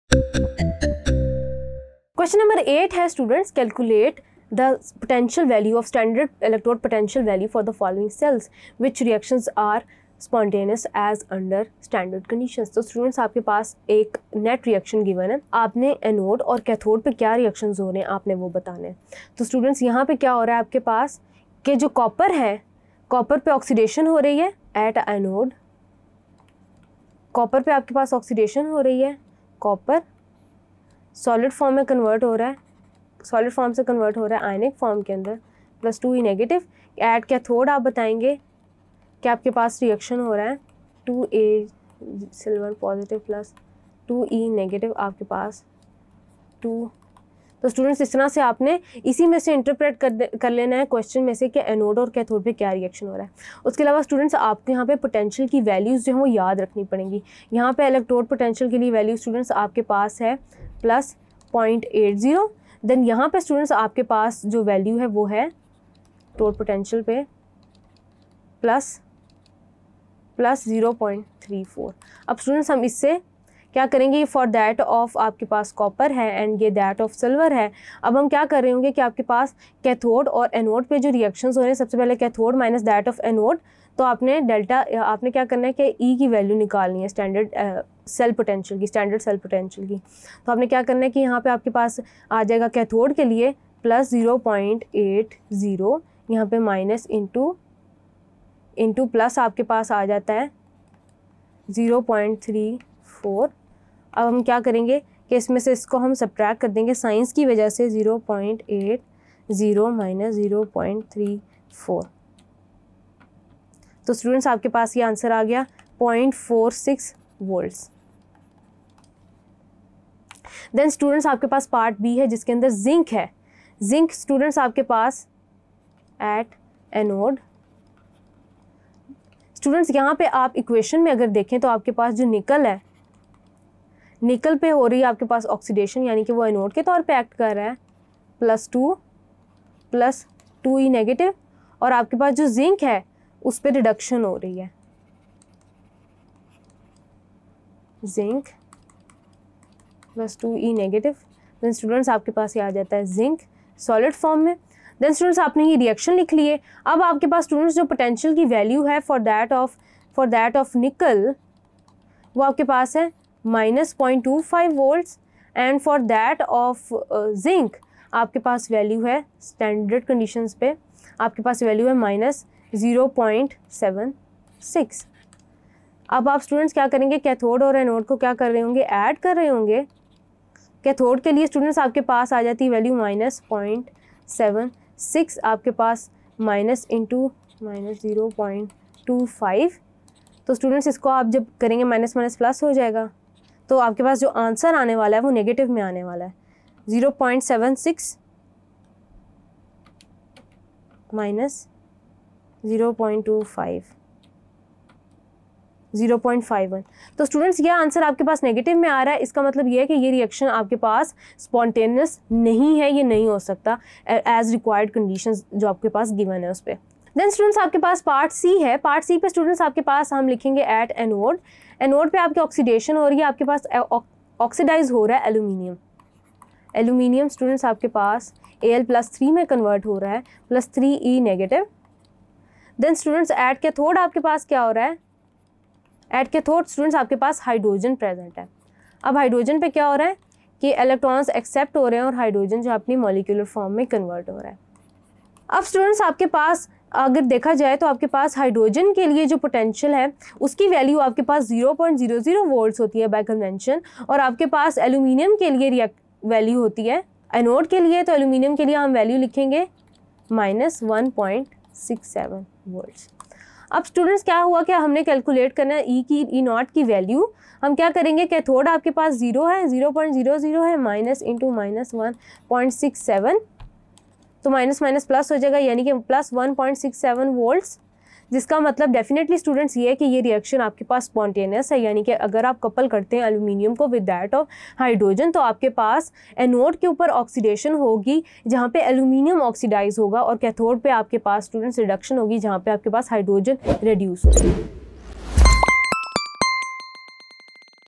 Chemistry Class 11 | Chapter 12 | Book Exercise 12.1 | Long Question 7 | in urdu | tutoria.pk
Let’s take a look at the Chapter 12 of Chemistry of Grade 11 “Electrochemistry”.

The teacher starts the chapter by defining Electrochemistry as the branch of science which deals with the conversion of electrical energy to chemical energy. Throughout the video, the instructor will talk about the practical examples of electrochemistry that you can witness in daily life.

In this video, you will study Chemistry Grade 11 Chapter 12 Topic 11 "Book Exercise 12.1 | Long Question 7" by instructor Rimsha Sehar.